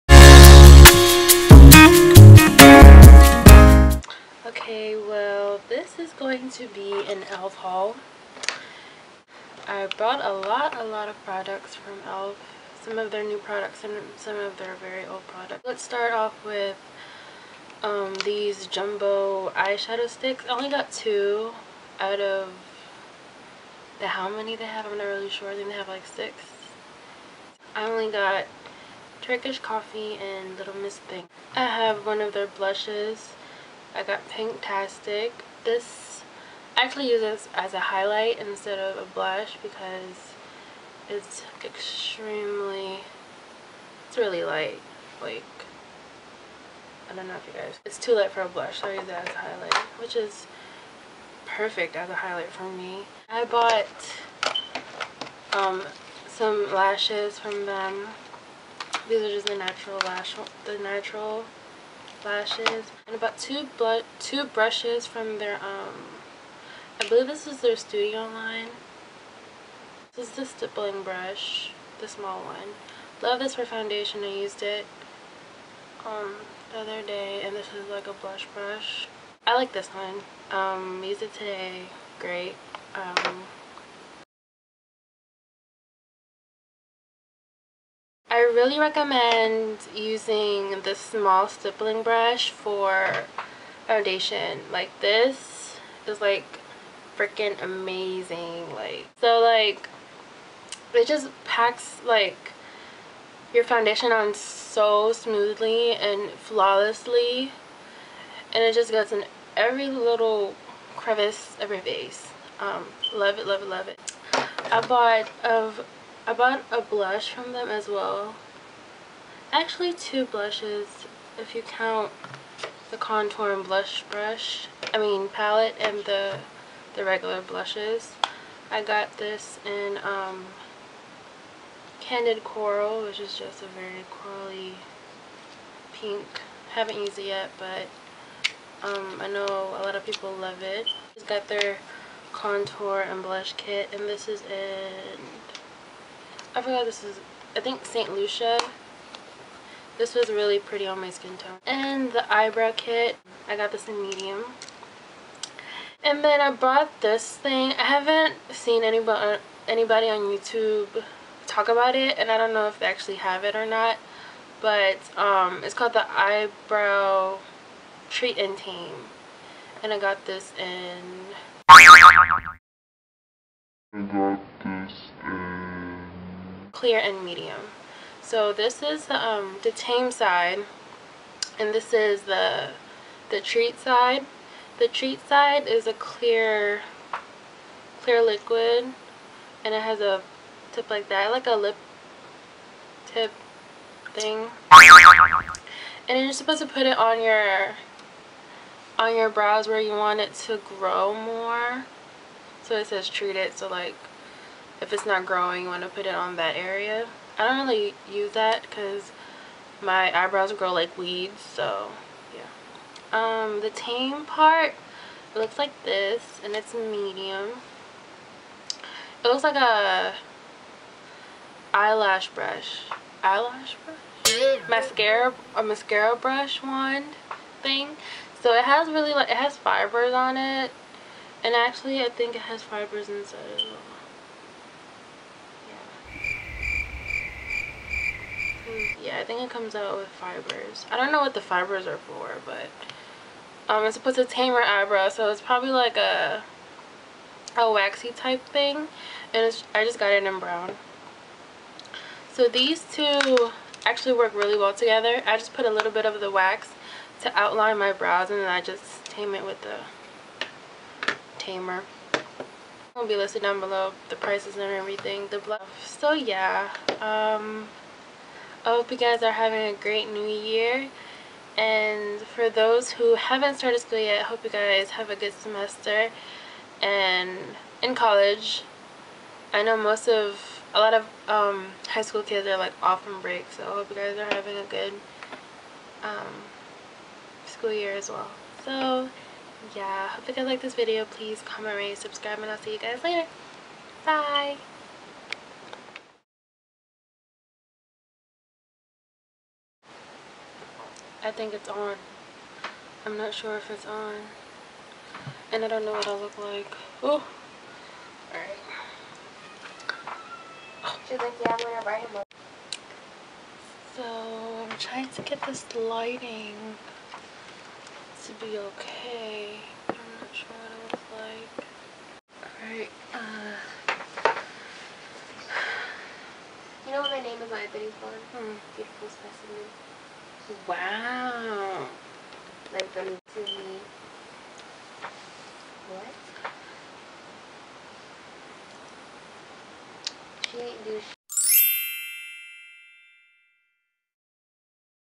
ELF Haul + Mini Review!
***READ ME!!***

Products Reviewed and Prices:

Jumbo Eyeshadow Sticks: $2 each
Blush in Candid Coral: $3
Bronzer & Contour Palette in St. Lucia: $3
Eyebrow Kit in Medium: $3
Eyebrow Treat & Tame in Medium: $3
False Lashes: $1 each
Baked Blush in Pinktastic: $3
Small Stippling Brush: $3
Blush Brush $3

WHAT'S ON MY..

Face:
Meyballine Fit Me 220 Natural Beige
Meyballine Fit Me 220 Powder
Milani Baked Blush in Rose D'oro
Sedona Lace Concealer Palette concealer

Eyes:
ELF Eyeshadow Stick in Little Miss Thing
Urban Decay Twice Baked in the Outer corner
Milani Liquif-eye in 01 Black
Meyballine Illegal Lengths Macara in VERY BLACK Waterproof
ELF Natural False Lashes

Lips:
EOS Lip Balm
Revlon Lip Butter in Creme Brûlée